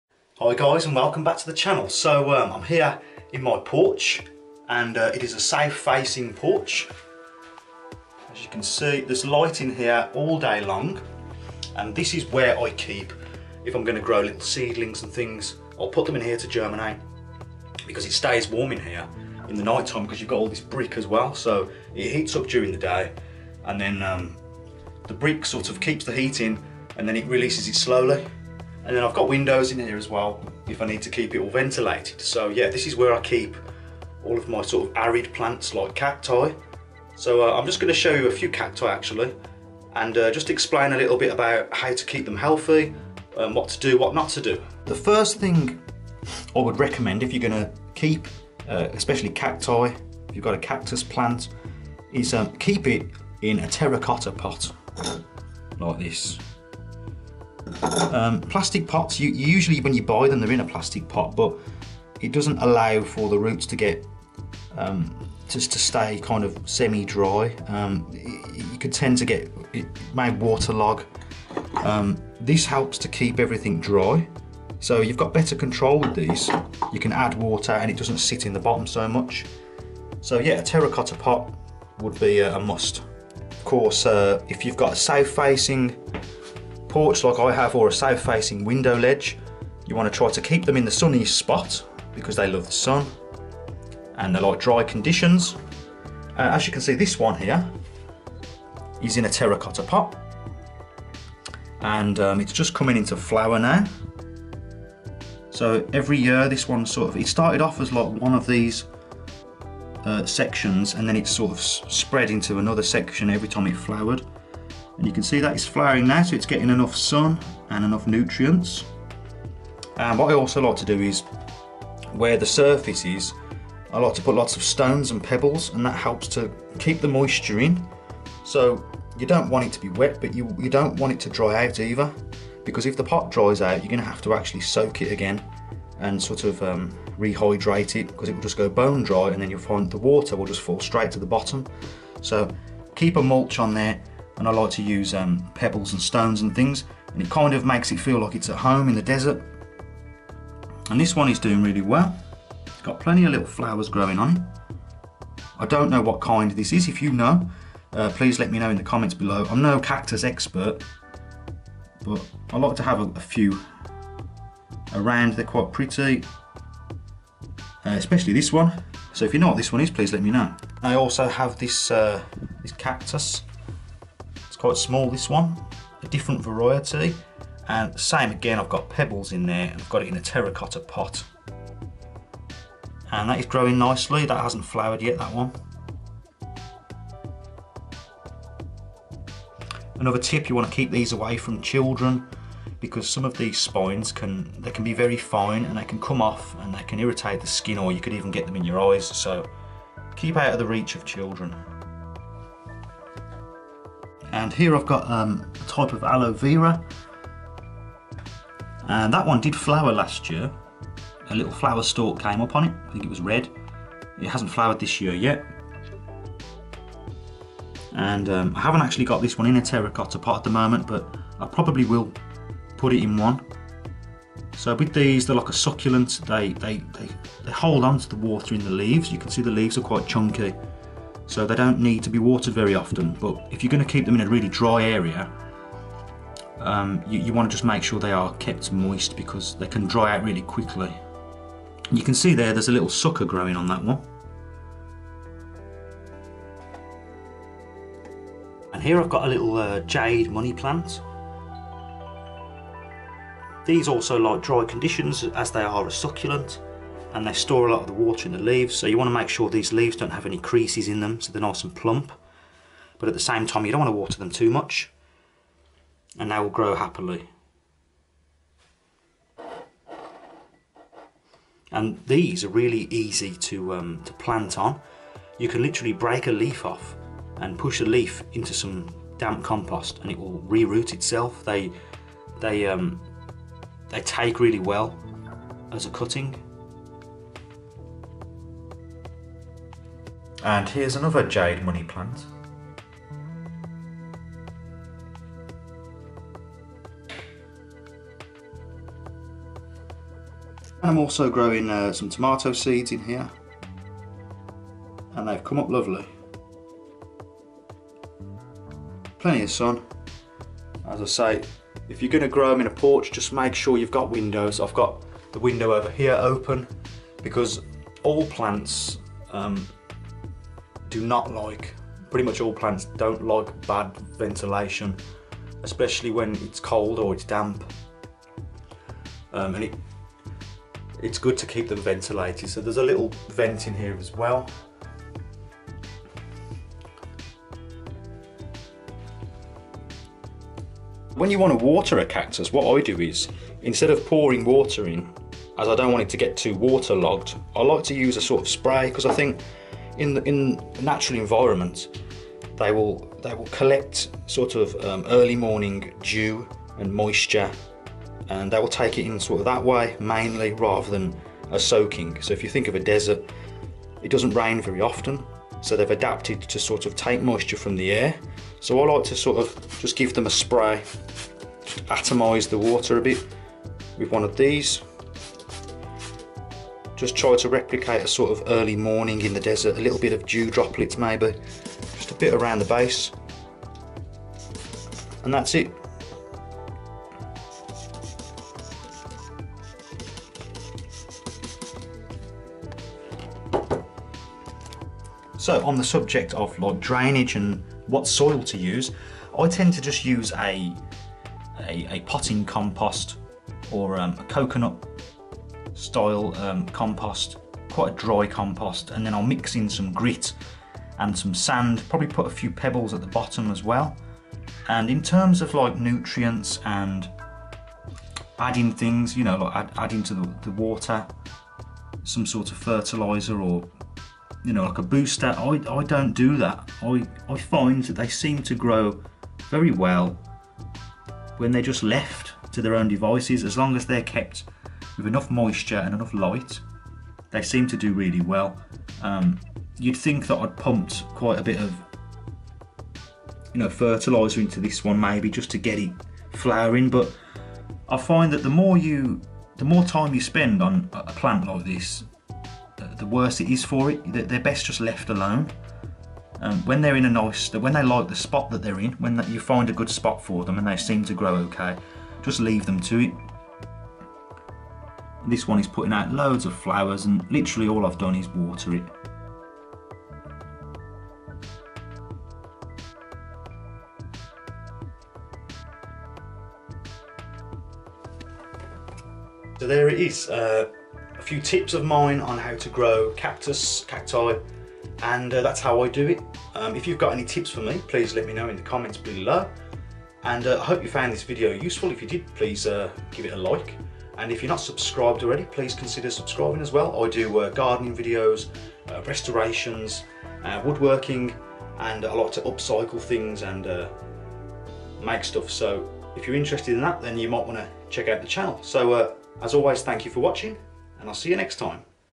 How to Care for and Grow Cacti / Cactus & Succulents - Cactus Care Tips
Just a quick Tip Video on how to care for and grow Cacti, Succulents, and other arid Plants. It is not an exact science although this is how I care for them and It works just fine for me.

0:00 Introduction
0:48 Terracotta Pots
3:28 Aloe vera
5:54 growing season.